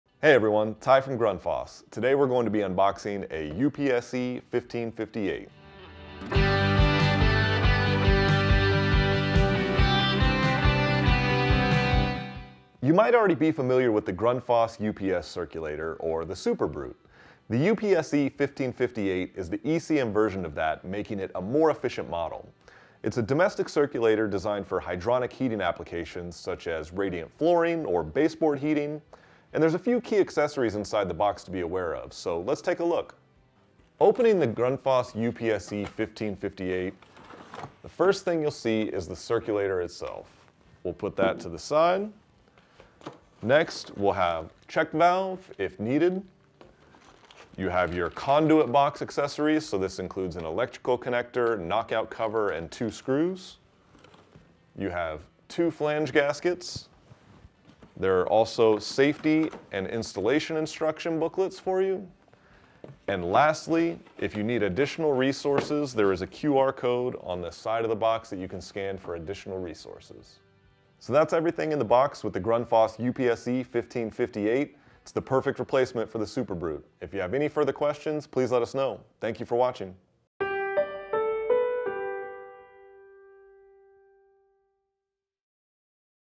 Unboxing a Grundfos UPSe 15-58
The Grundfos UPSe 15-58 is a high-efficiency circulator fitted with an electronically commutated motor (ECM). It is designed to circulate liquids in heating and cooling systems and offers one of the highest Energy Ratings in the market (193) based on the Hydraulic Institute Energy Rating.

The pump features a wide range of controls, including constant pressure, proportional pressure, and constant curve, each with three settings, making it suitable for almost any hydronic application. The UPSe meets the energy conservation standards for circulator pumps the Department of Energy established, which will take effect in May 2028.